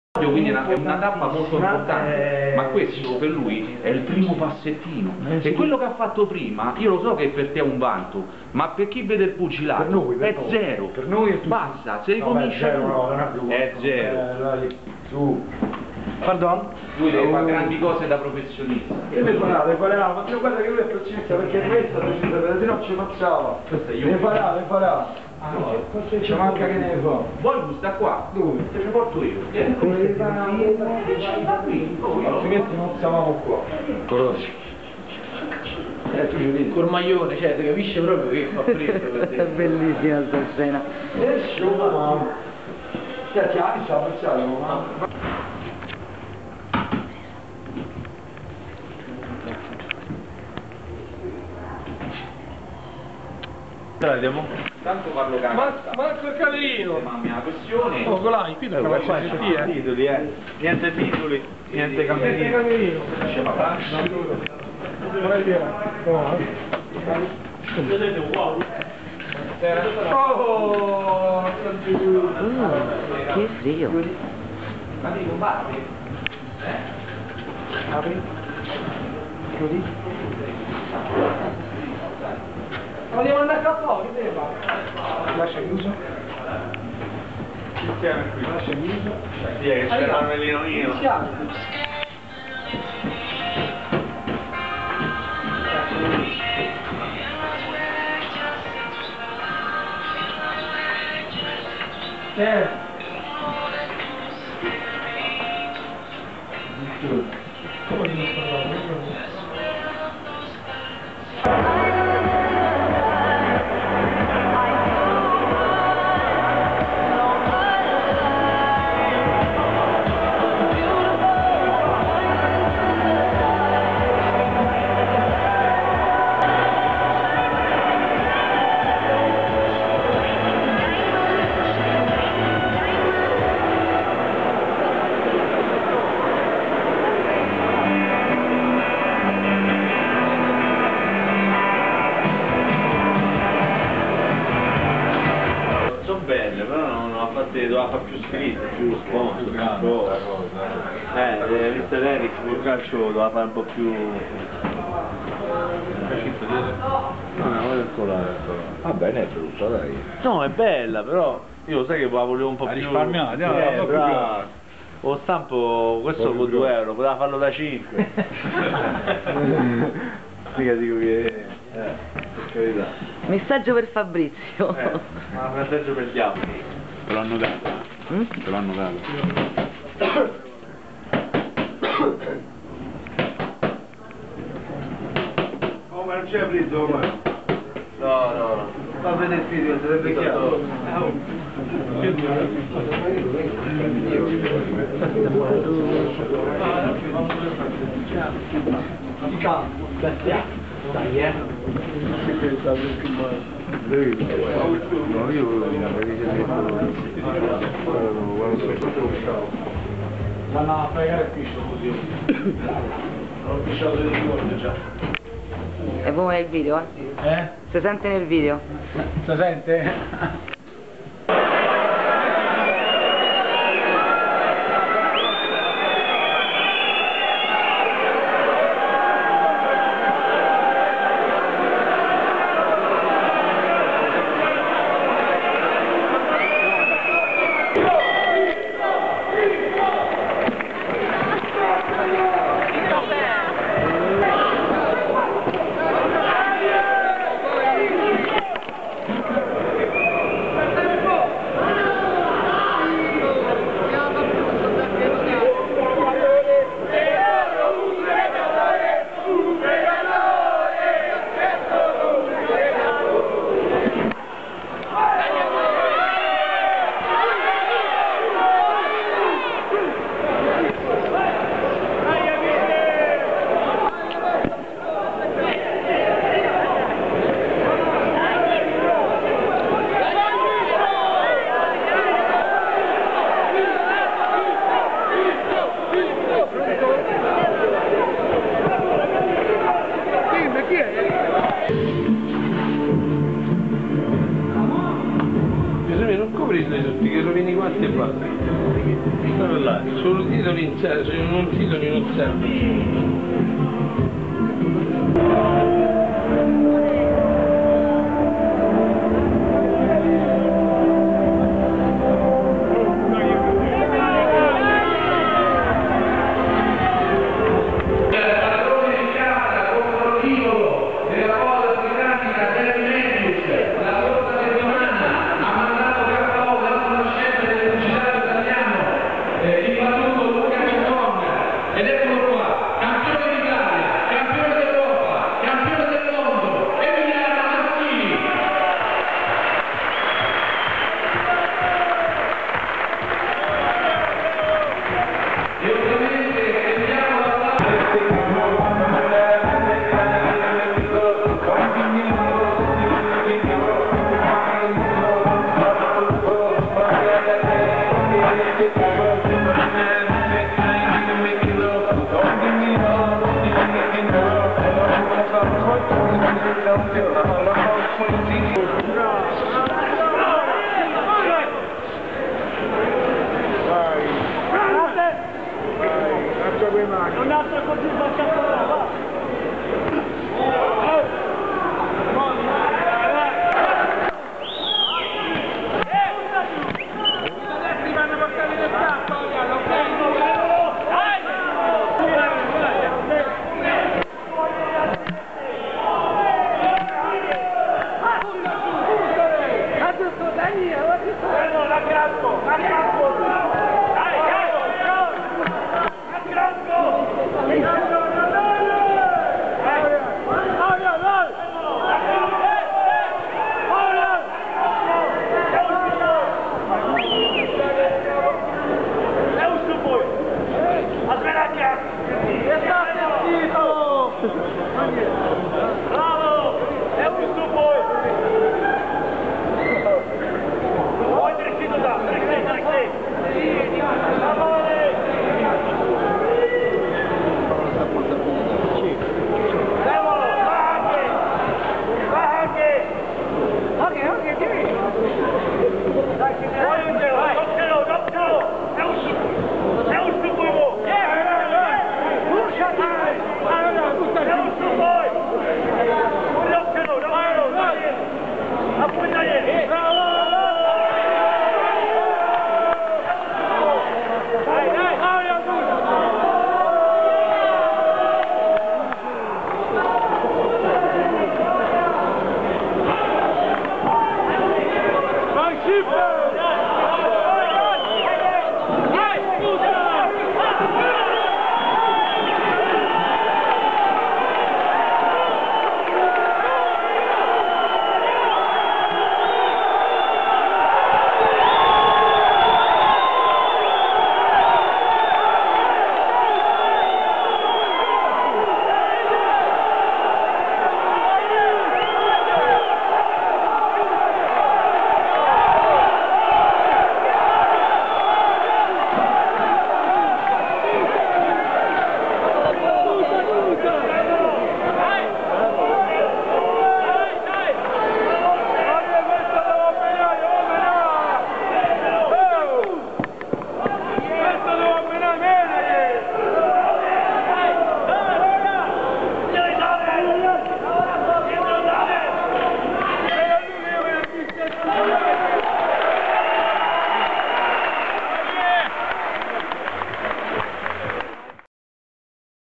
Serata di boxe BBT a Roma
Orial Kolaj  vs Jindrich Velecky
Massimiliano Buccheri  vs Attili Kiss
Mirco Ricci vs Fouad Nasri
Manuel Lancia  vs Michael Escobar
Valerio Ranaldi vs Stelian Voicu